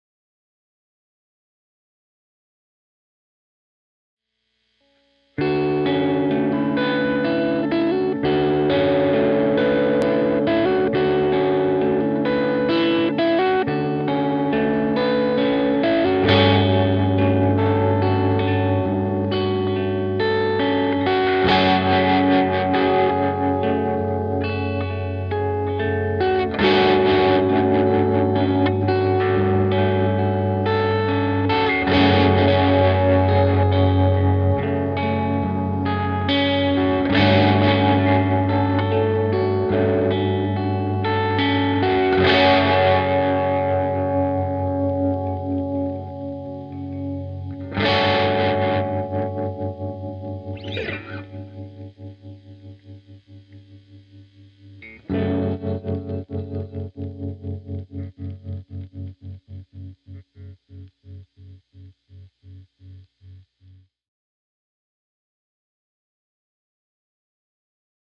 Concurso pedal Tremolo Spurr Audio
Video con un pequeño riff de mi autoría para participar en el concurso por un peda Spurr Audio - Laundr O' Matic por medio de Pepis Music!

Equipo utilizado:
-Guitarra B.C Rich KK Beast V
-Caline Highway Man (Overdrive) - Biyang
-Interfaz Behringer XENYX Q502USB